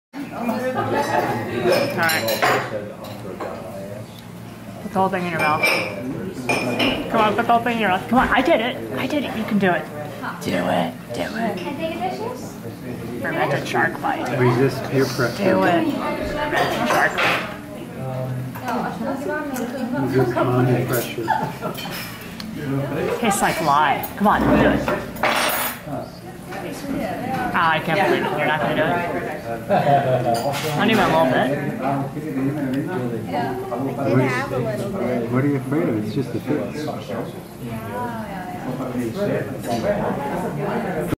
Leo eating a small bite of fermented, rotted Greenland shark (Hákarl)
Mom tries to pressure Leo into eating the whole piece!

Fresh Greenland shark is poisonous when fresh, because of high levels of urea and trimethylamine oxide. So you have to let the shark fully rot to get rid of the toxins. You dig a hole in the sand and bury the shark for a couple months and then you dry it. Hákarl has a lot of ammonia and thus smells like floor cleaner. I wish I had known beforehand that I was supposed to wash it down with the local grain alcohol.